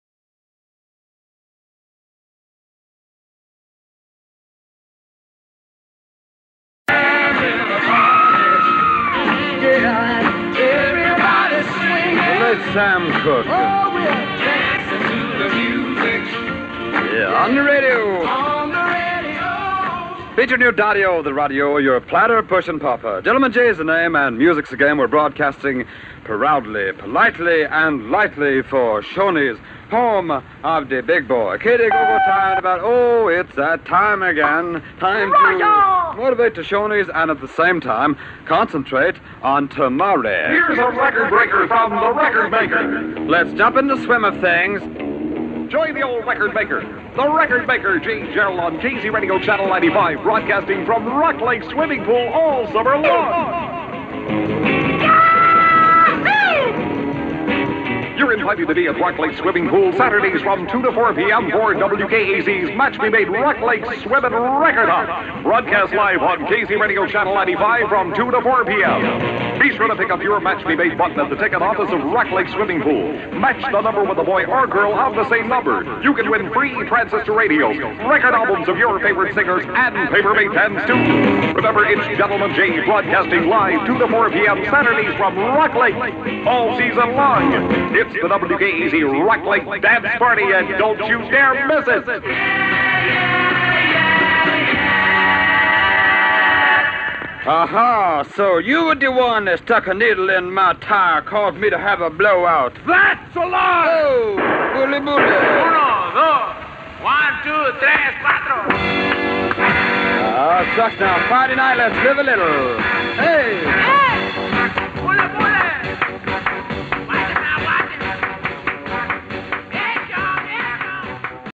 Poppa Jay Jarrell and a Rock Lake Commercial in the 60s.
The Daddy-O-Of -The-Radio Poppa Jay Jarrell and a Rock Lake Commercial in the 60s.